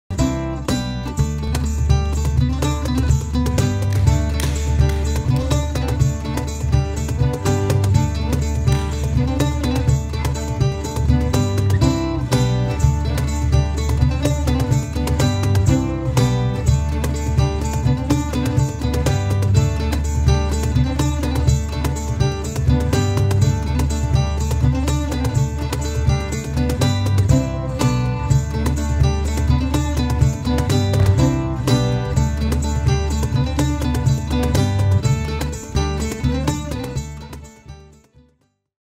Volttitanssi 2017 (Pari)
Kalentanssi 2017. Koreografia Susanna Kivinen ja Kaisa Alatalo. Musiikin sovitus: Joonas Ojajärvi. Kalenat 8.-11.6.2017 Lappeenranta.